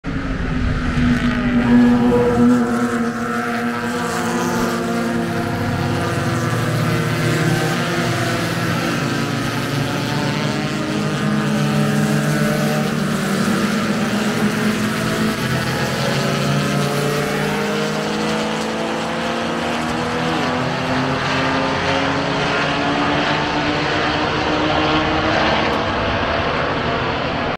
PCA Start 2022 DJI Pocket 2
Just an example of the PCA club racing at Road AMerica and what a start looks like. Many fine cars.